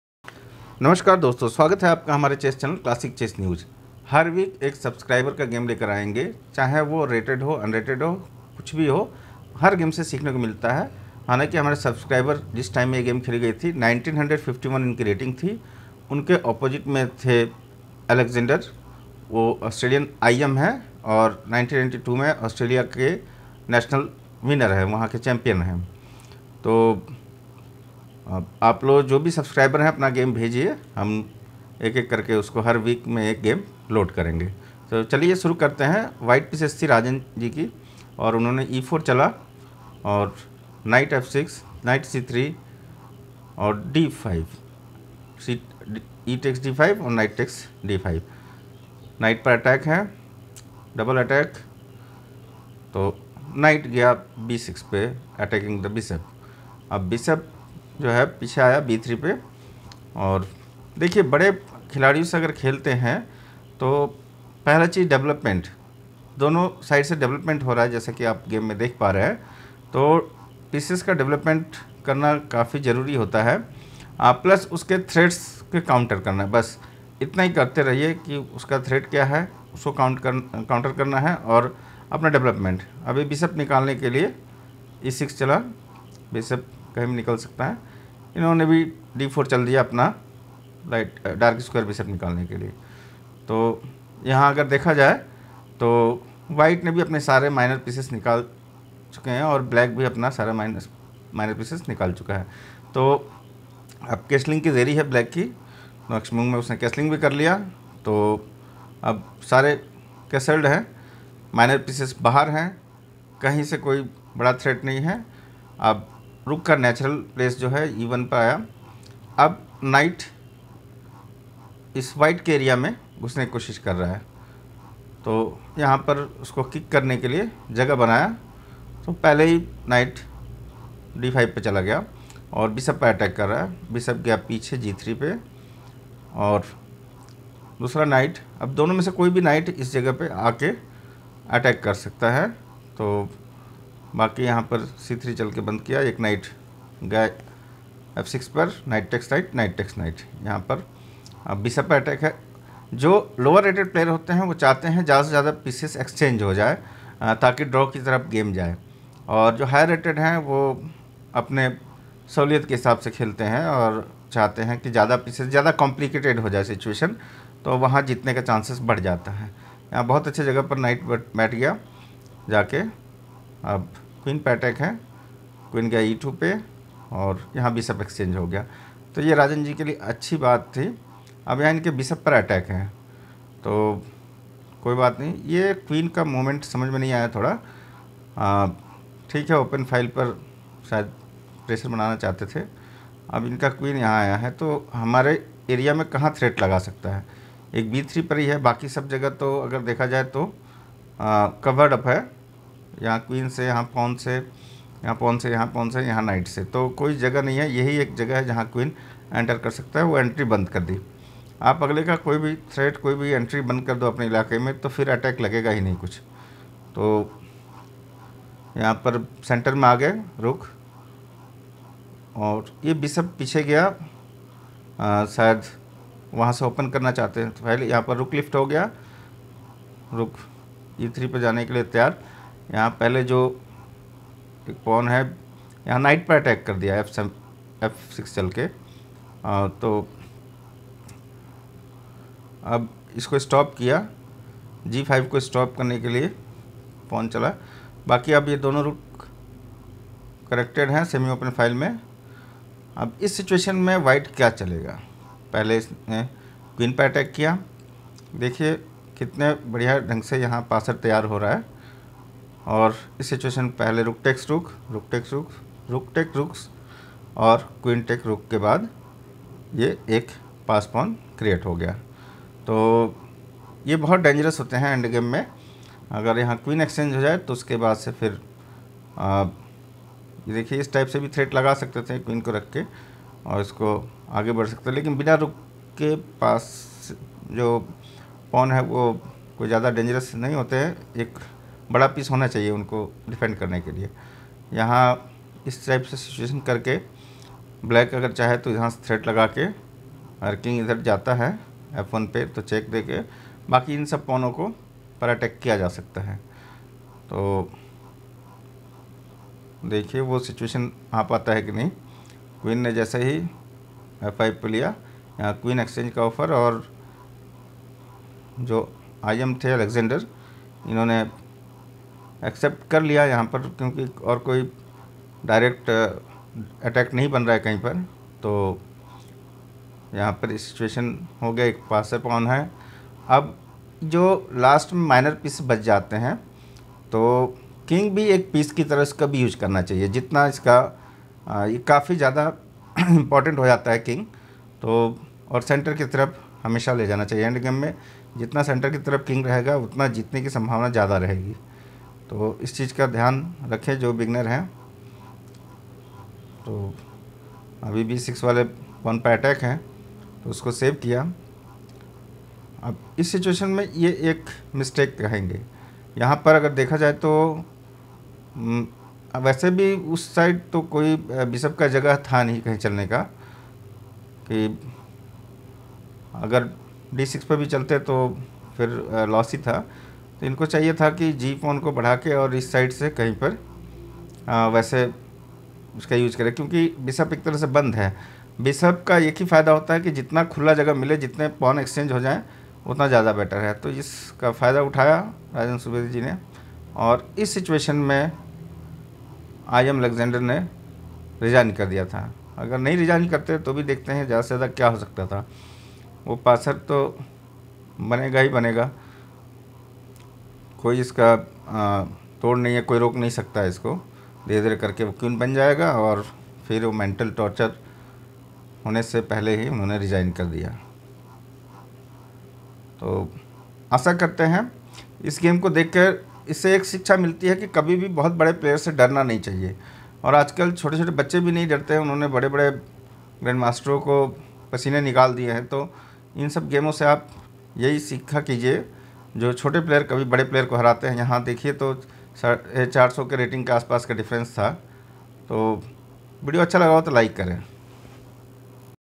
How a low rated player beat a Champion IM rating above 2400 - Subscribers game - 1
Rajan Subedi (Nepal) Rating 1951 and Wohl Aleksander (Australia) Rating 2411